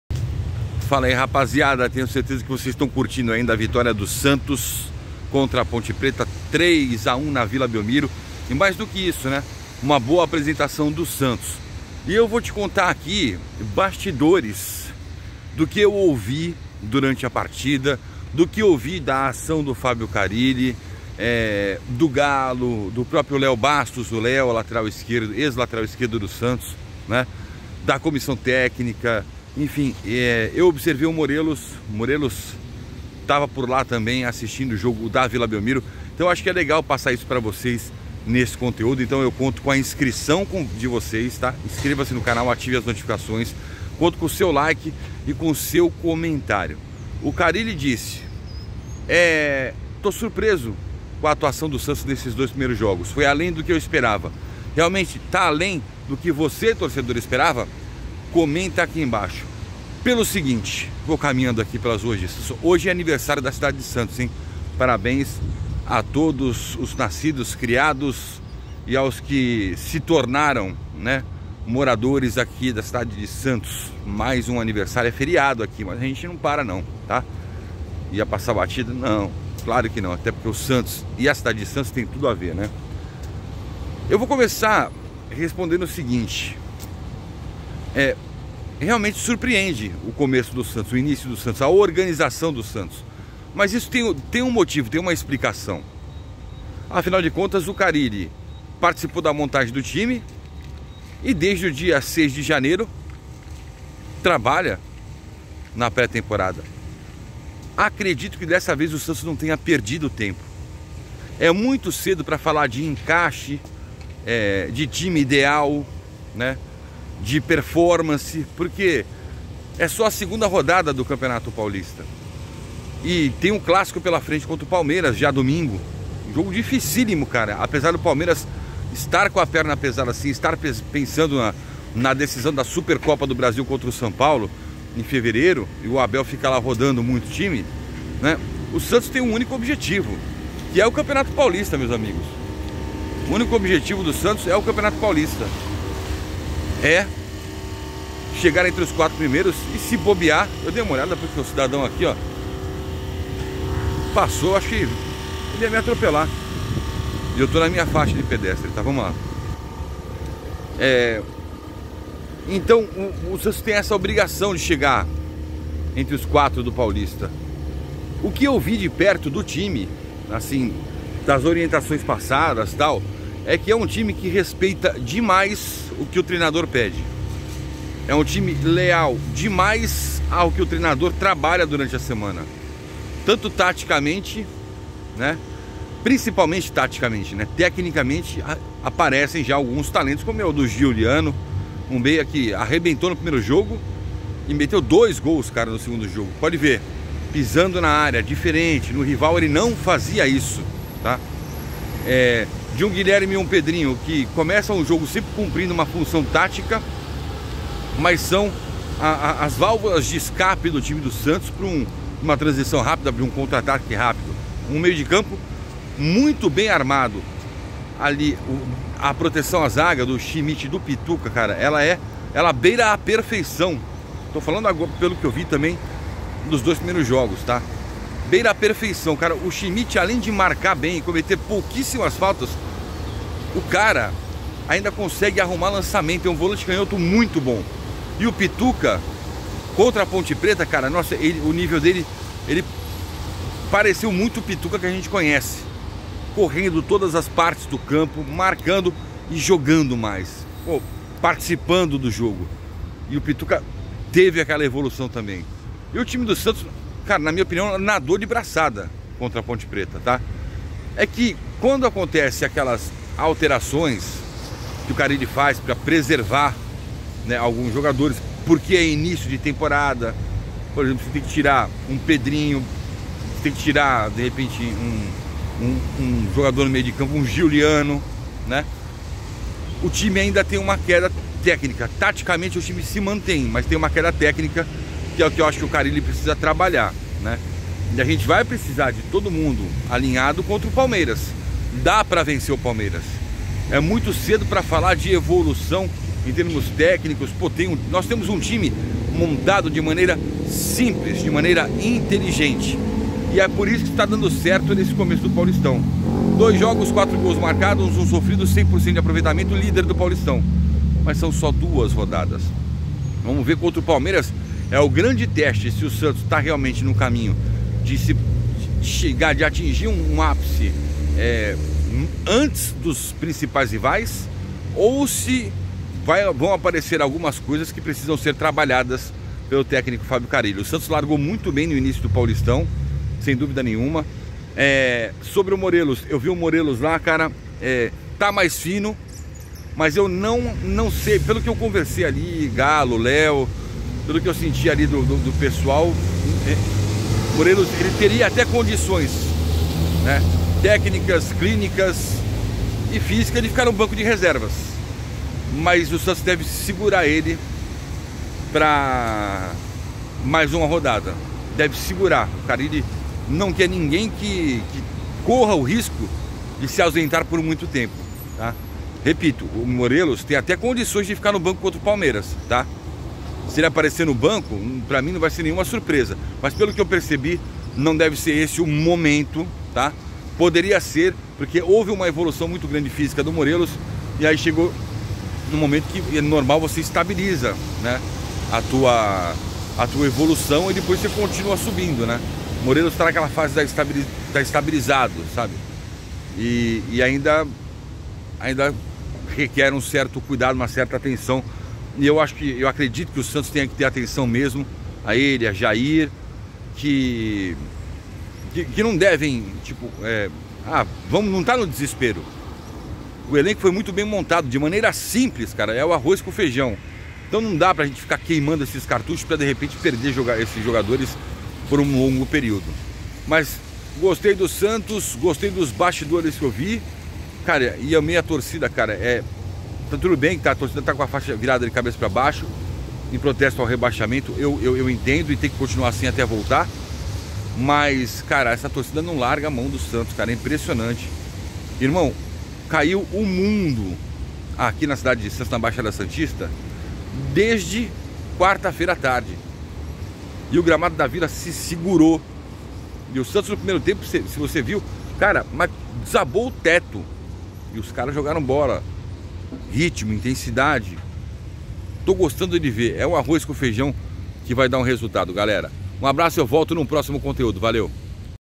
COMO ASSIM? CARILLE FALA QUE NÃO ESPERAVA O ALTO RENDIMENTO DO SANTOS NO INÍCIO DO PAULISTÃO ! VEJA!
NOVOS CANAIS

E SÓ DÁ SANTOS - OFICIAL

RADIO ESPORTE NEWS FM - TODOS OS GOLS DA RODADA DO BRASILEIRÃO